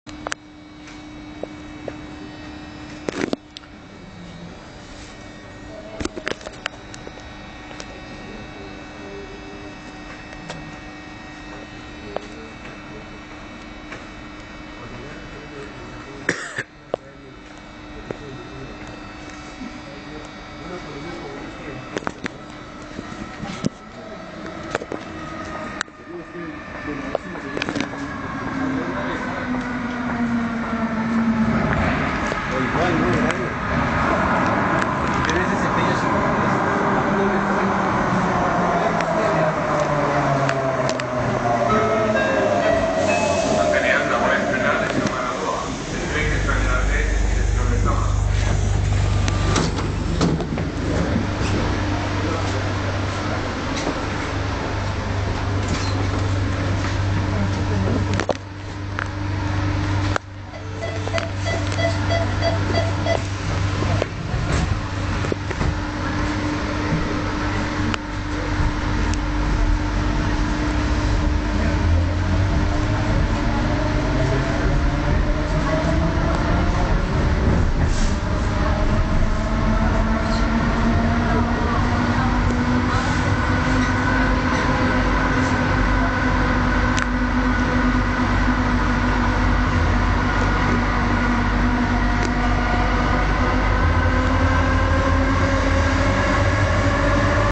Euskotren viaje en la 304 en Casco Viejo. Avería y retraso
Como se apreciaba en el video hubo alguna avería y por ello se escuchaba por la Megafonía en la Estación de Casco Viejo

La unidad 304 Procedente de Deusto con Destino Lezama